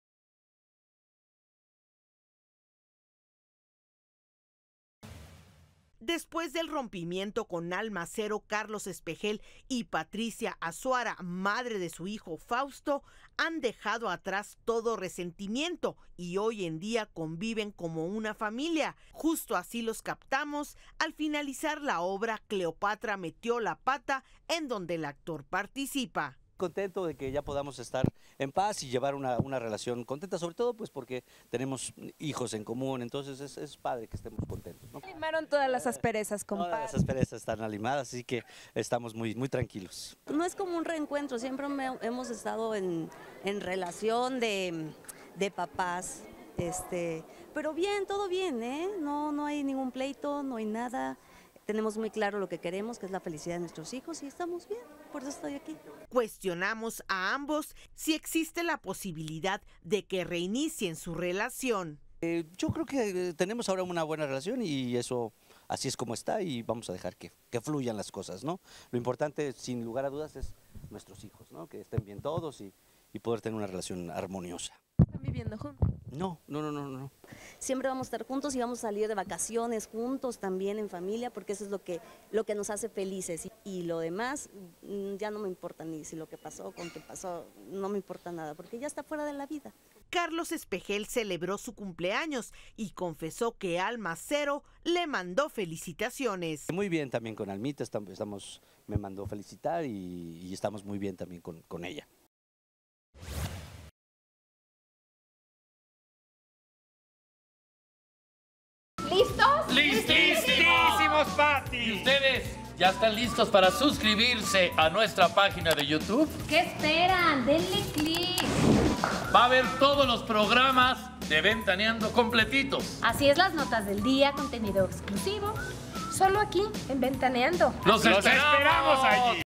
¿Carlos Espejel terminó su relación con Alma Cero y se reconcilió con la madre de su hijo?
El actor afirma que limaron asperezas, dejaron atrás todo resentimiento y conviven como familia.